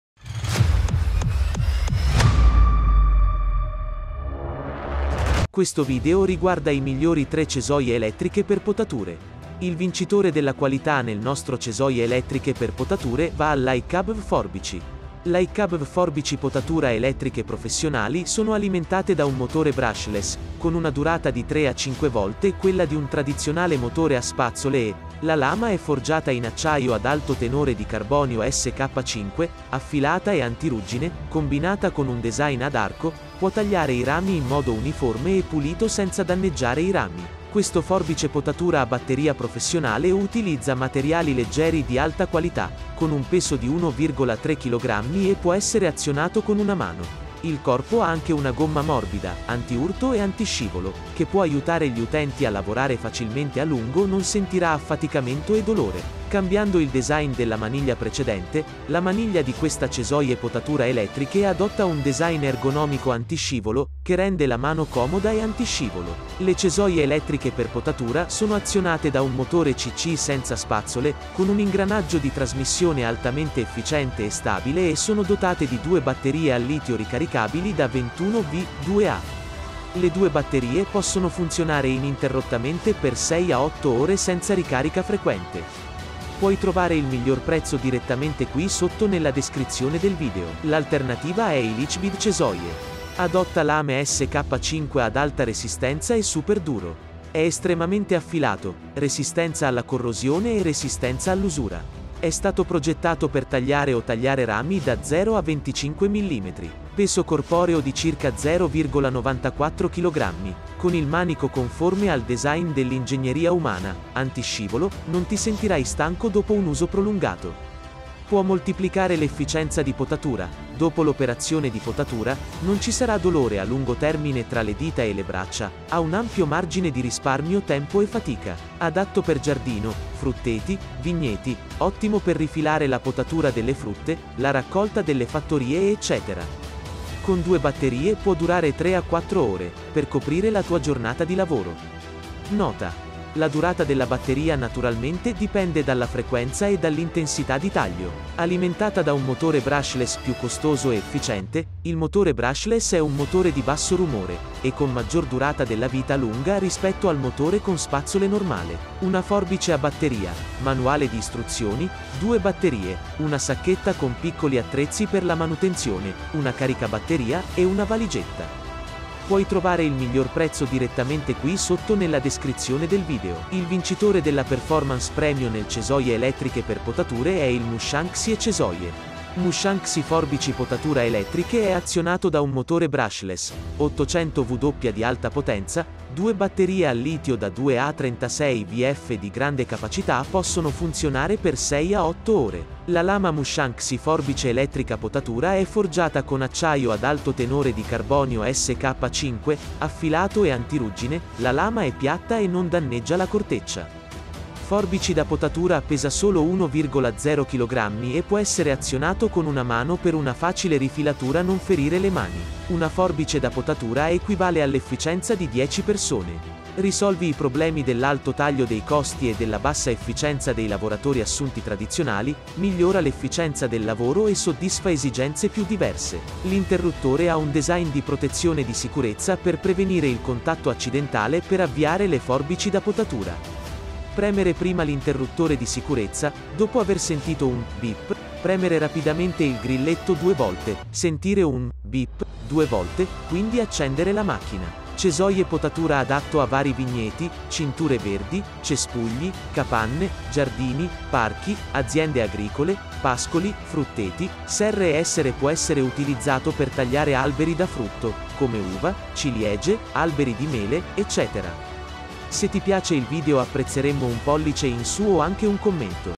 ➤ Le 3 migliori cesoie elettriche per potature ✓
Questo video riguarda i migliori 3 cesoie elettriche per potature.

Il vincitore della qualità nel nostro cesoie elettriche per potature va al Laecabv Forbici.

Laecabv forbici potatura elettriche professionali sono alimentate da un motore brushless, con una durata di 3-5 volte quella di un tradizionale motore a spazzole e. La lama è forgiata in acciaio ad alto tenore di carbonio SK5, affilata e antiruggine, combinata con un design ad arco, può tagliare i rami in modo uniforme e pulito senza danneggiare i rami.
Questo forbice potatura a batteria professionale utilizza materiali leggeri di alta qualità, con un peso di 1,3 kg e può essere azionato con una mano.Il corpo ha anche una gomma morbida, antiurto e antiscivolo, che può aiutare gli utenti a lavorare facilmente a lungo Non sentirà affaticamento e dolore.
Cambiando il design della maniglia precedente, la maniglia di questa cesoie potatura elettriche adotta un design ergonomico antiscivolo, che rende la mano comoda e antiscivolo.
Le cesoie elettriche per potatura sono azionate da un motore CC senza spazzole, con un ingranaggio di trasmissione altamente efficiente e stabile e sono dotate di 2 batterie al litio ricaricabili da 21 V/2 Ah. Le 2 batterie possono funzionare ininterrottamente per 6-8 ore senza ricarica frequente.

L'alternativa è il Eachbid Cesoie.

Adotta lame SK5 ad alta resistenza e super duro. È estremamente affilato, resistenza alla corrosione e resistenza all'usura. È stato progettato per tagliare o tagliare rami da 0 a 25 mm.
Peso corporeo di circa 0,94 kg, con il manico conforme al design dell'ingegneria umana, antiscivolo, non ti sentirai stanco dopo un uso prolungato. Può moltiplicare l'efficienza di potatura, dopo l'operazione di potatura, non ci sarà dolore a lungo termine tra le dita e le braccia, ha un ampio margine di risparmio tempo e fatica.
Adatto per giardino, frutteti, vigneti; ottimo per rifilare la potatura delle frutte, la raccolta delle fattorie e ecc. Con 2 batterie può durare 3-4 ore, per coprire la tua giornata di lavoro. Nota: La durata della batteria naturalmente dipende dalla frequenza e dall'intensità di taglio.
Alimentata da un motore brushless più costoso e efficiente, il motore brushless è un motore di basso rumore, e con maggior durata della vita lunga rispetto al motore con spazzole normale.
una forbice a batteria, manuale di istruzioni, 2 Batterie, una sacchetta con piccoli attrezzi per la manutenzione , una caricabatteria, e una valigetta.
Il vincitore della performance premio nel cesoie elettriche per potature è il Mtianxy e Cesoie.

Mtianxy Forbici Potatura Elettriche è azionato da un motore brushless, 800 W di alta potenza, due batterie al litio da 2 Ah 36 VF di grande capacità possono funzionare per 6-8 ore.
La lama Mtianxy Forbice Elettrica Potatura è forgiata con acciaio ad alto tenore di carbonio SK5, affilato e antiruggine, la lama è piatta e non danneggia la corteccia. Forbici da Potatura pesa solo 1,0 kg e può essere azionato con una mano per una facile rifilatura Non ferire le mani.
Una forbice da potatura equivale all'efficienza di 10 persone. Risolvi i problemi dell'alto taglio dei costi e della bassa efficienza dei lavoratori assunti tradizionali, migliora l'efficienza del lavoro e soddisfa esigenze più diverse.
L'interruttore ha un design di protezione di sicurezza per prevenire il contatto accidentale per avviare le forbici da potatura. Premere prima l'interruttore di sicurezza, dopo aver sentito un "bip", premere rapidamente il grilletto due volte, sentire un "bip" due volte, quindi accendere la macchina.
Cesoie Potatura Adatto a vari vigneti, cinture verdi, cespugli, capanne, giardini, parchi, aziende agricole, pascoli, frutteti, serre e essere può essere utilizzato per tagliare alberi da frutto, come uva, ciliegie, alberi di mele, ecc.